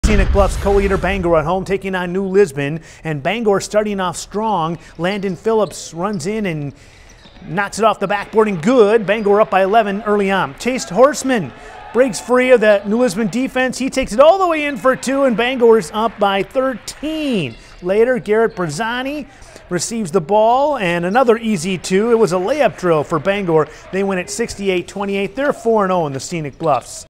Bangor Boys Basketball stomp New Lisbon 68-28
Bangor hosted New Lisbon Thursday night in a one-sided affair.